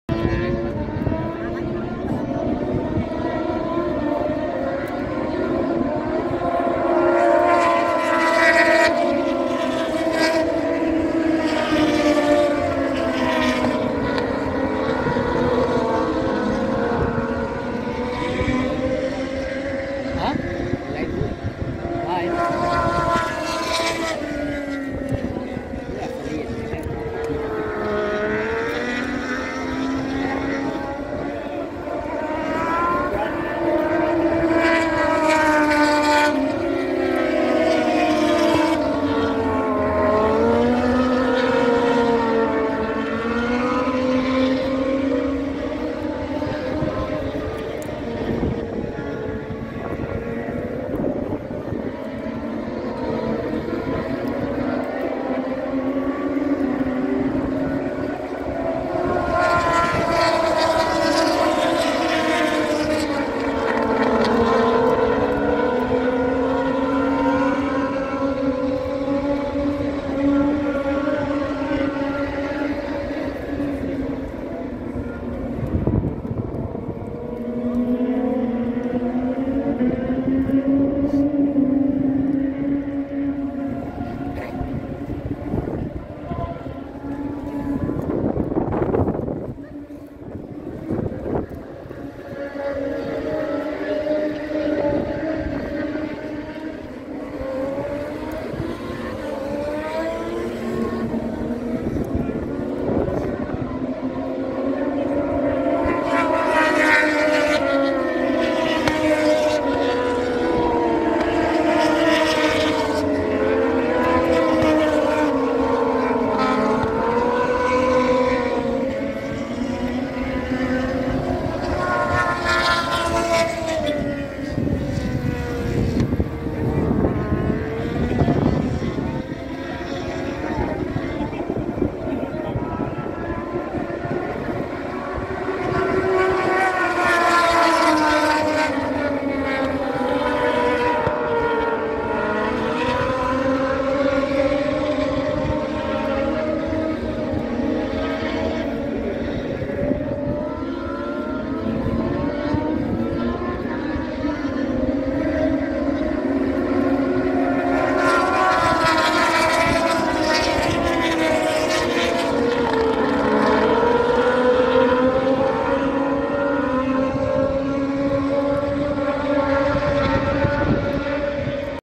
Qualifikasi 1 Moto2 Sirkuit Mandalika lombok
Sesi qualigikasi  pertama moto2 sirkuit mandalika pada hari ke 2.
Moto2, motogp mandalika, sirkuit mandalika, lombok, kuta mandalika.